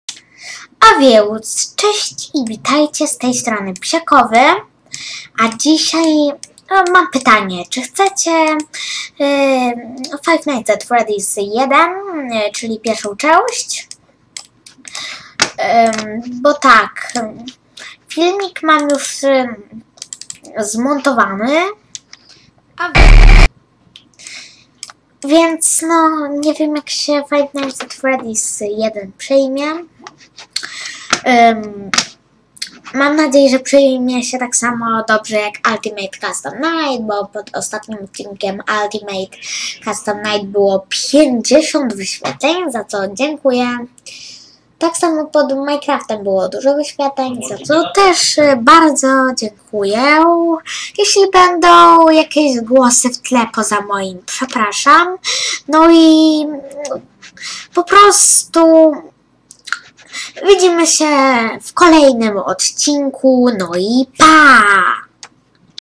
Pytanie
Chcecie Five Night’s At Freddy’s , bo nie wiem jak się przyjmie